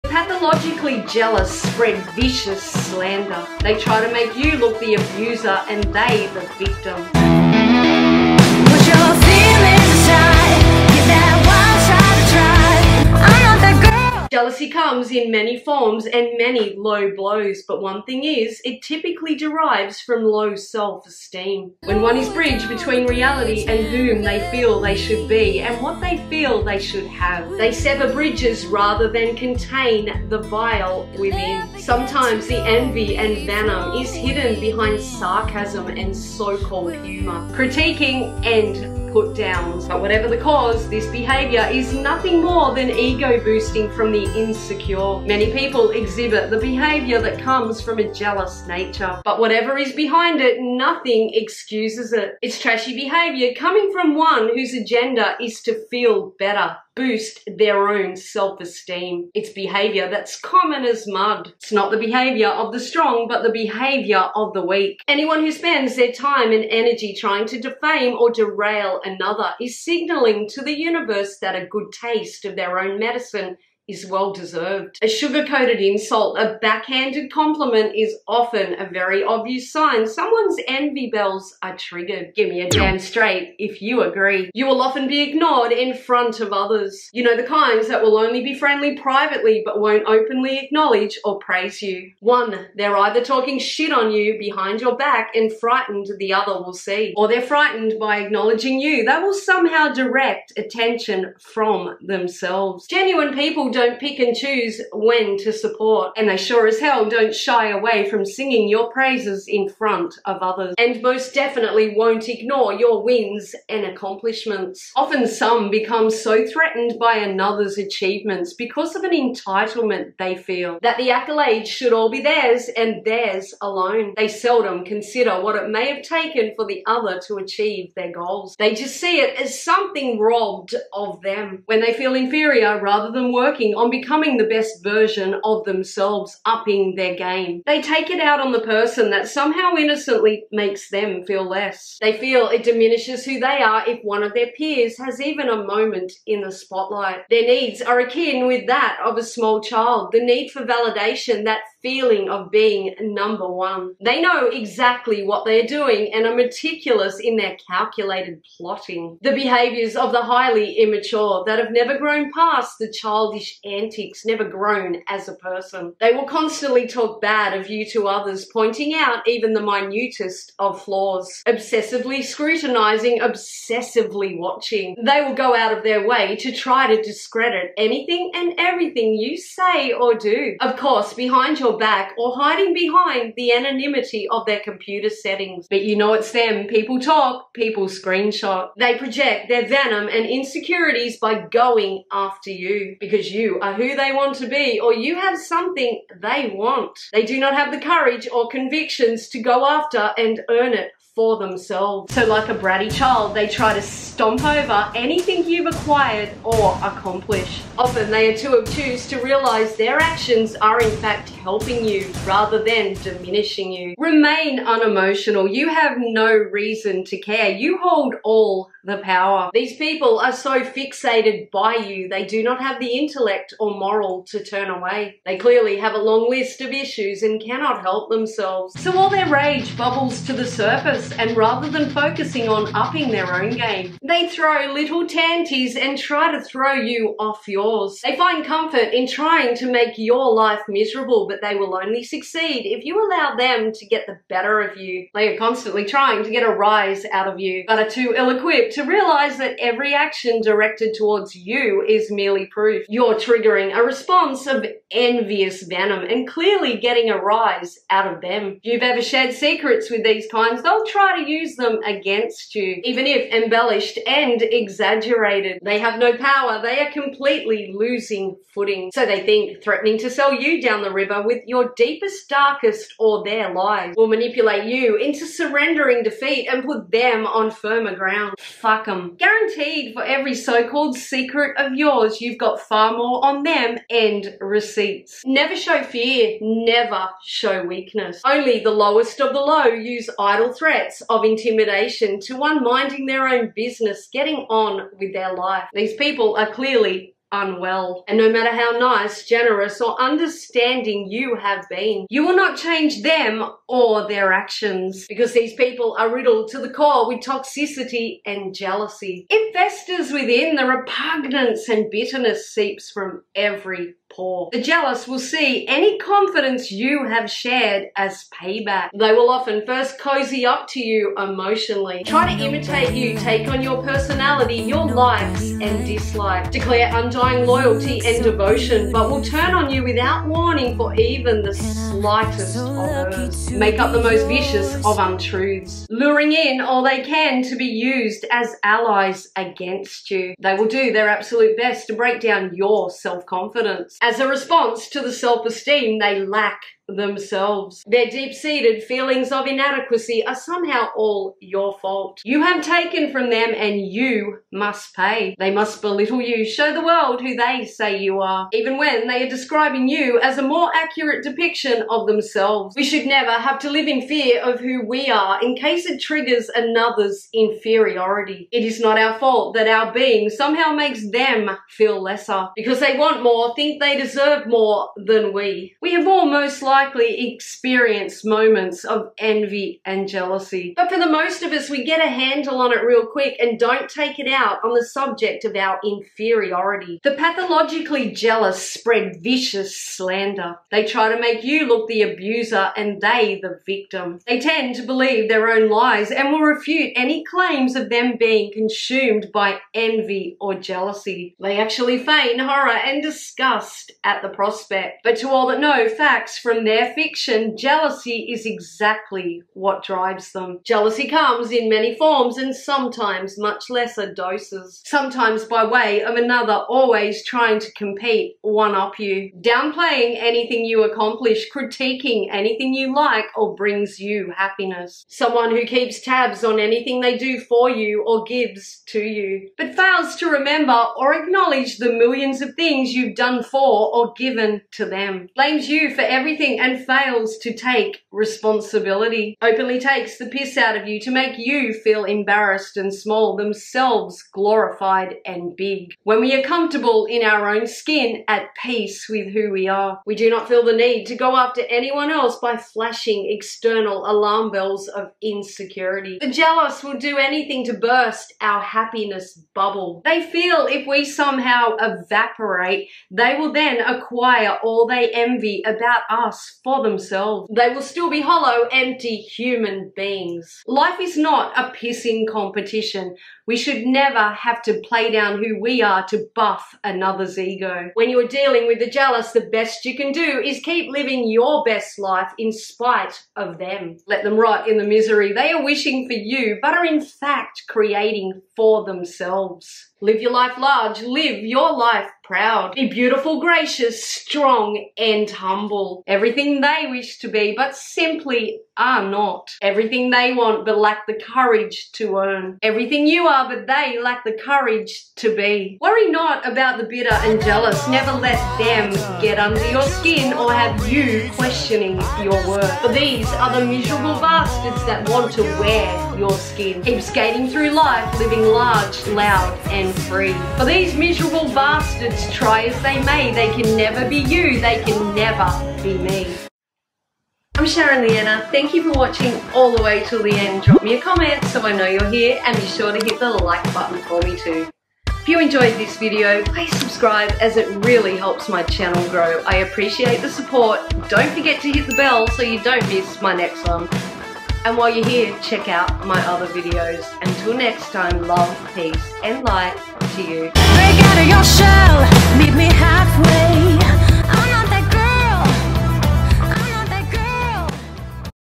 How To Deal With Jealous People! | Signs Someone Is Jealous Of You!
How to deal with jealous people..Signs someone is jealous of you! Envious! So many are consumed by jealousy and envy..

But how do we keep these kinds from ruining our own inner peace!

Many people exhibit the behaviours that come from a jealous nature..

This video is a a candid unfiltered video from my own perspective on how to spot signs of the jealous and how to deal with it.

Jealousy and envy comes in many guises..

We've most likely all experienced these emotions during our lifetime but most of us can get a handle on it real quick..

If you are dealing with jealous people are a victim of jealousy and envy then this video may give you a new perspective on how to cope..

❤️ Sharon H

And be sure to check my other video’s whilst you’re here ❤️

Playlists below!!! ⤵️